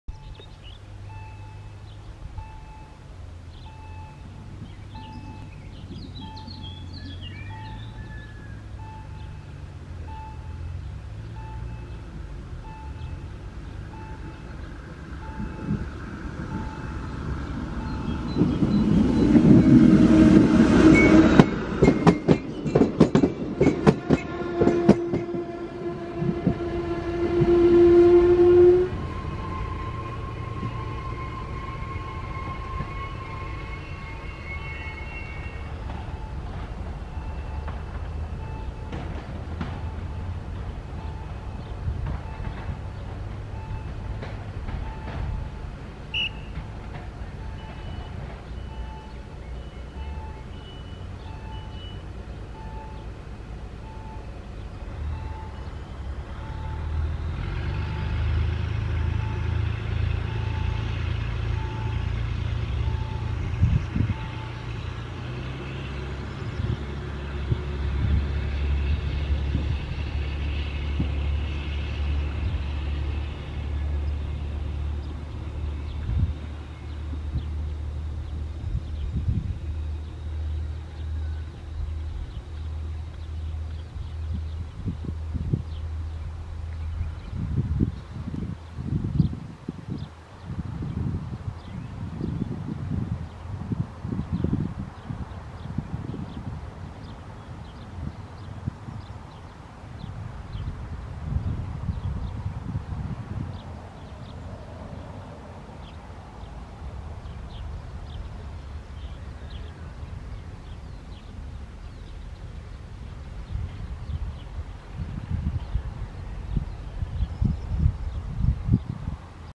SA108-004 R-85726 Krzyż- Chojnice
SA108-004 jako R-85726 Krzyż- Chojnice wjeżdża na przystanek osobowy Siedlisko Czarnkowskie w dniu 16 maja 2017 r.